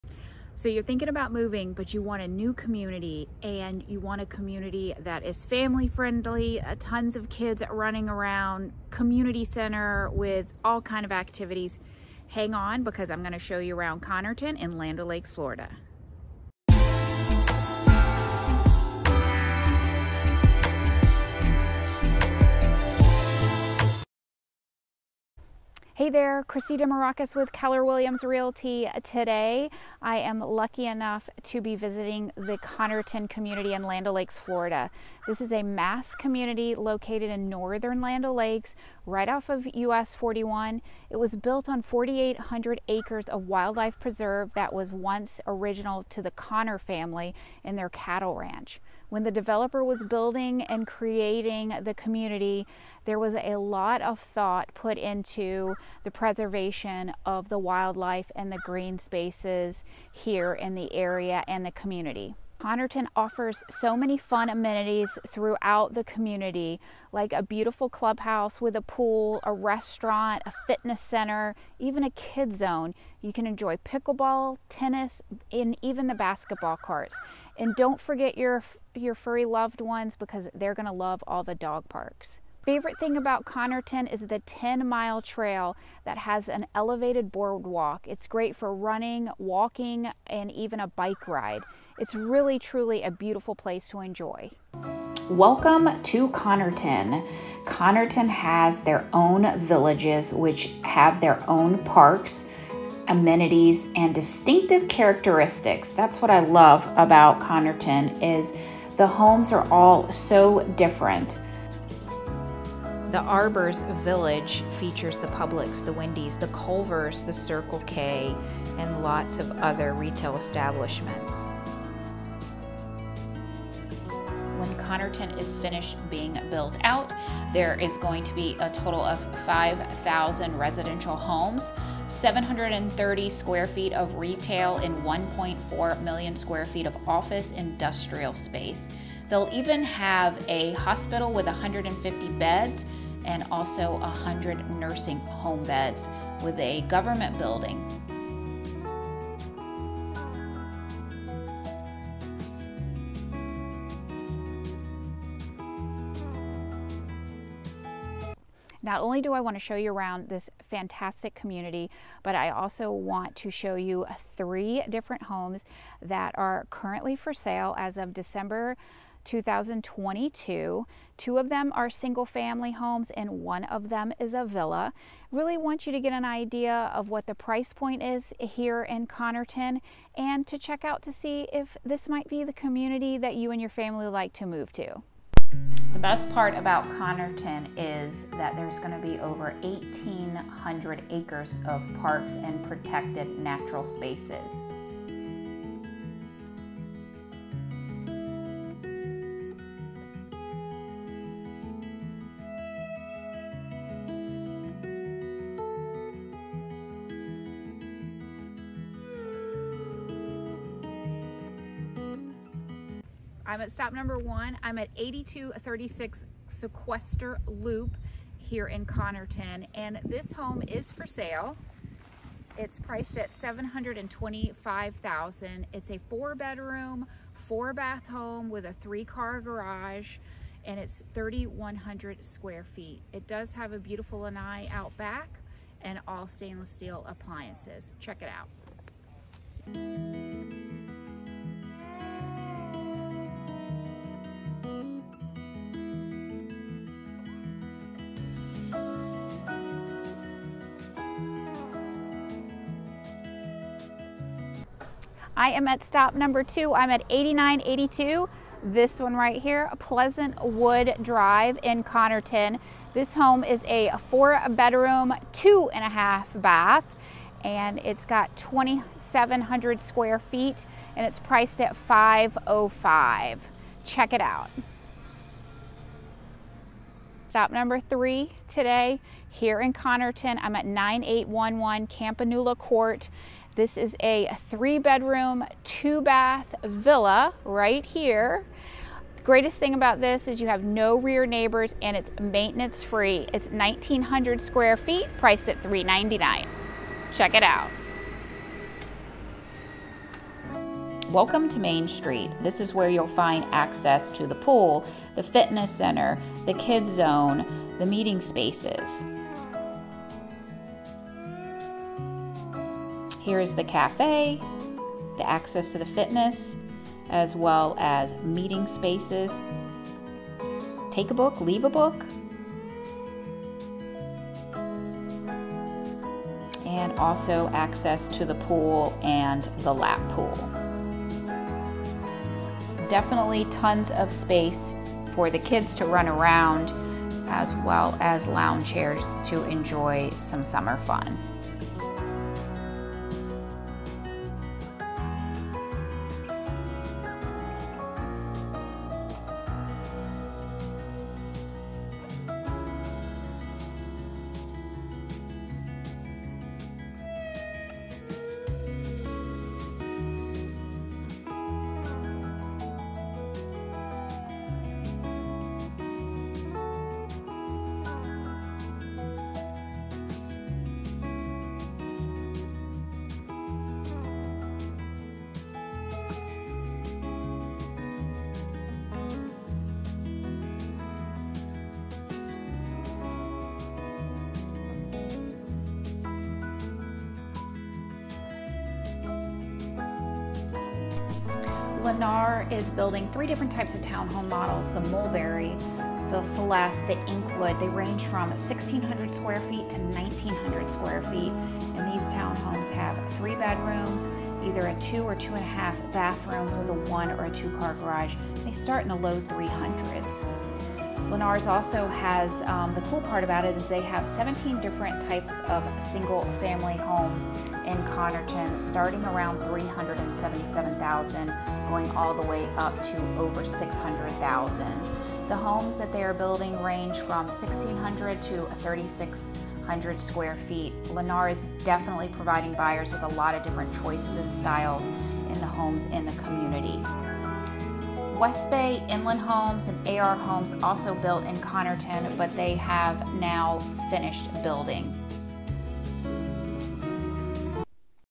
Moving to Land O Lakes Florida- Connerton
Connerton is a master planned community in Land O Lakes Florida right off of US 41. It's located about 25 miles North of Tampa & about an hour to the most beautiful Gulf coast beaches including Clearwater & St. Pete Beach.

Homes in this community range in price from about $300+k for a townhome to the low millions for a Luxury Estate home.

Nestled within a nature preserve in Pasco County, the community is home to many different types of wildlife.  There are miles of nature trails to explore and many amenities for the residents to enjoy!

Like to get a copy of my Relocation Guide?

 Looking to buy or sell? I can help with that!